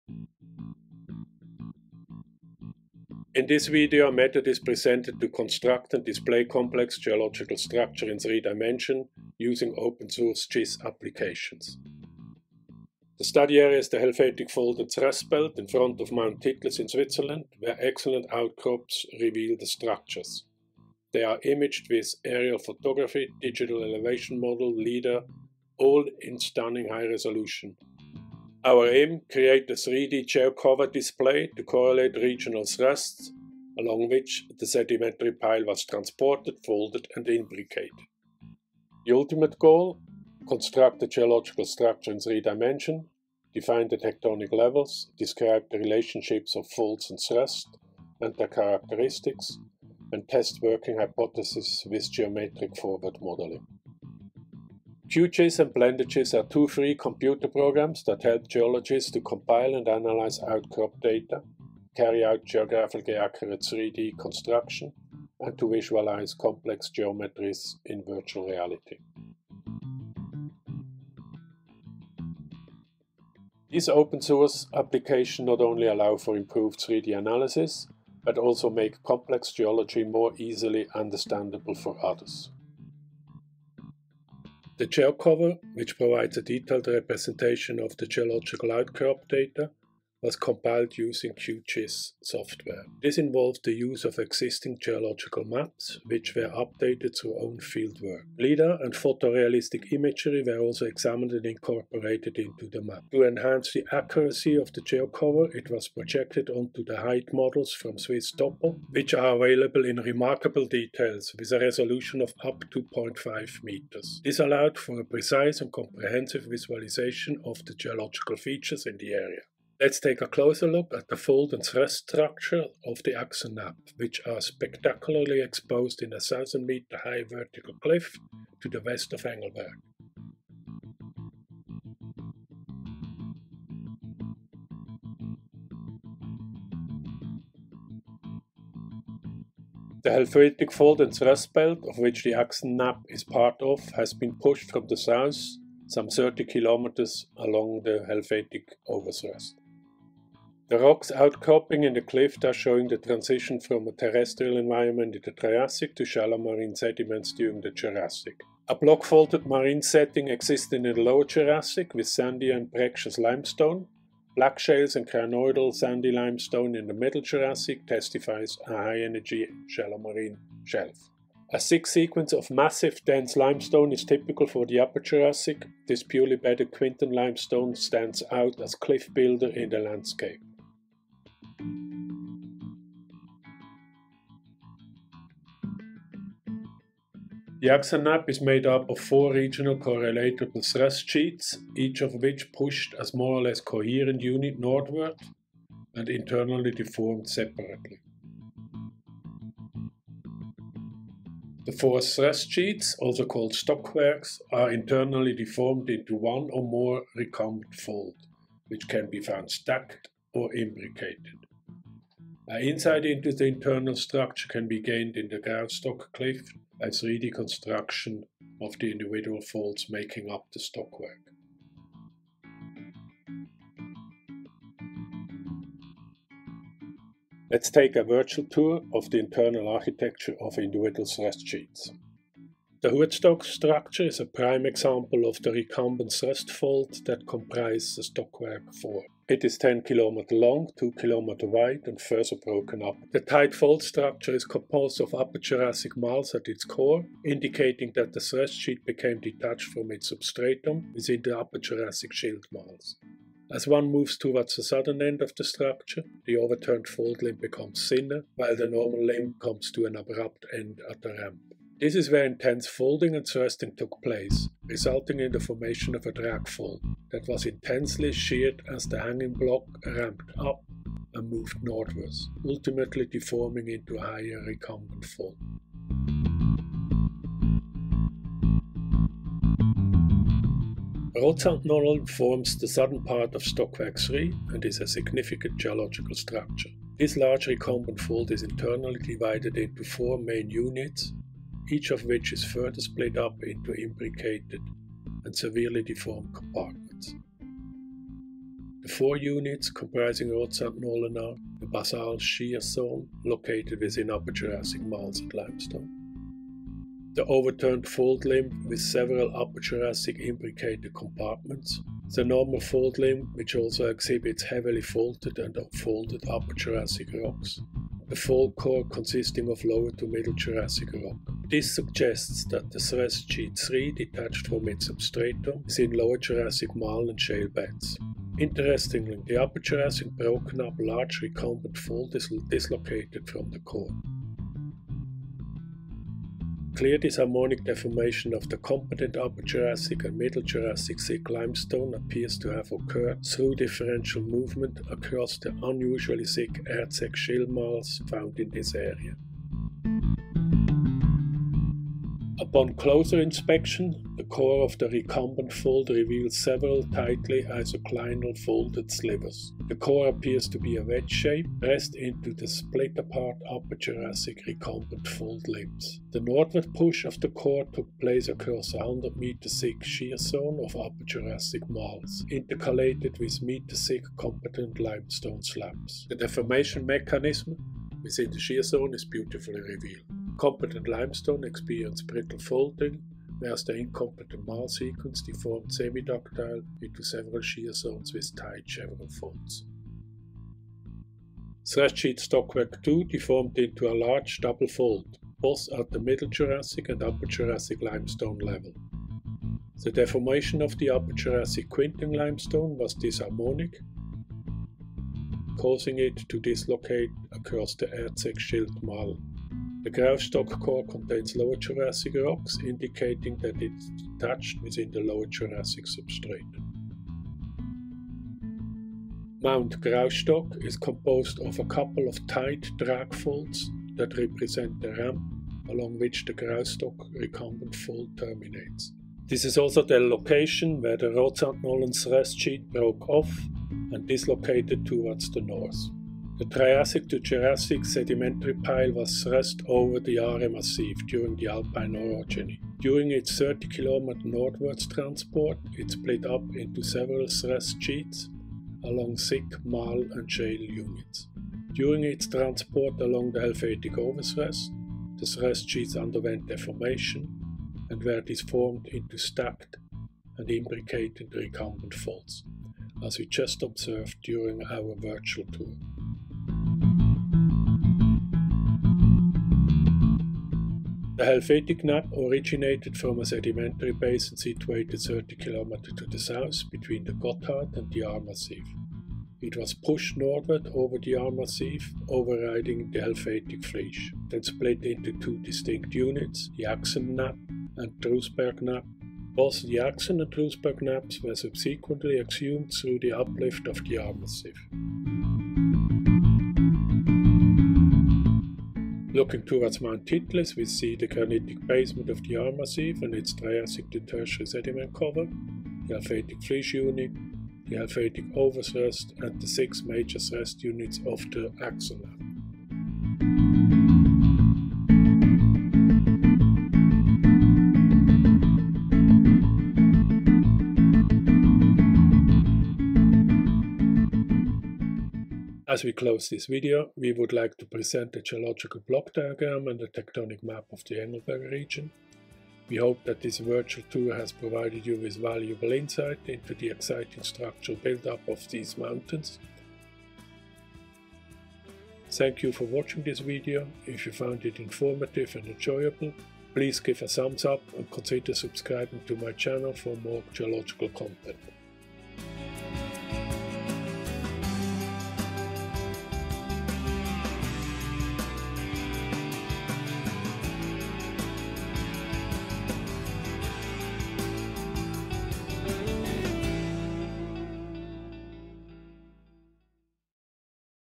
Insight into the Architecture of the Helvetic Fold & Thrust belt, Switzerland - A 3D Geological ...
In this video, we showcase a cutting-edge method for constructing and visualizing geological structures in 3D, using open-source GIS software. Specifically, we focused on the Helvetic fold and thrust belt in Switzerland, utilizing QGIS, BlenderGIS, and Google Earth, in conjunction with high-resolution LiDAR and photo-realistic imagery to create a 3D geological display that facilitates a deeper understanding of the interrelationships between folds and thrusts.
By projecting geological outcrop data onto "swisstopo" height models, we constructed a virtual reality model of the Helvetic fold and thrust belt, capturing the complex geometries of the six thrust sheets (tectonic stockwerk) and their internal structures in stunning detail. This approach not only enhances 3D analysis and comprehension of mountain architecture and deformational structures but also provides a more accessible and cost-effective method for geologists to explore and present complex geology.
In summary, our innovative approach using open-source, versatile GIS applications unlocks the potential to visualize and analyze the 3D architecture of mountain belts, providing valuable insights into the formation and evolution of these geological features.